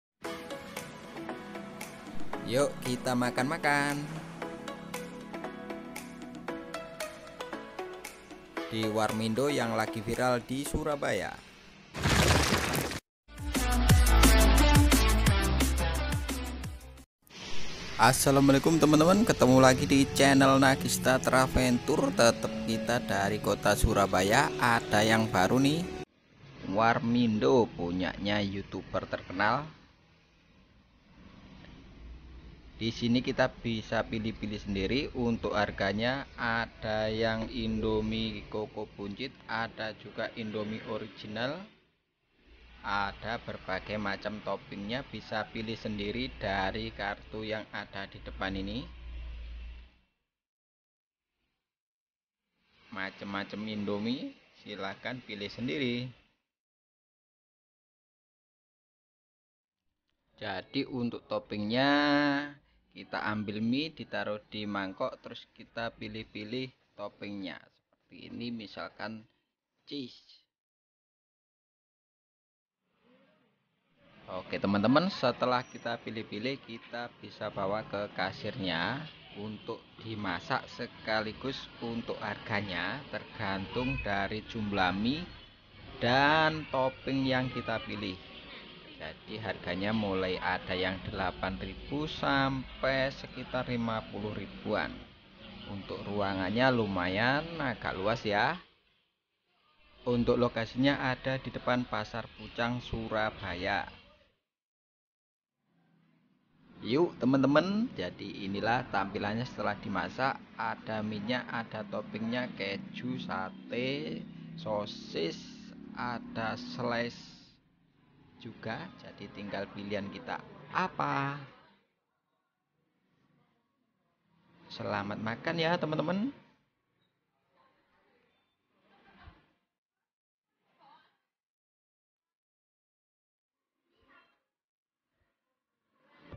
Warmindo sultan di surabaya || harganya mahal tapi topingnya buanyak....
Warmindo emang sudah banyak ada dimana-mana. Tapi yang satu ini berbeda dengan yang lainnya. Dari harga dan topingnya berbeda dari warmindo pada umumnya.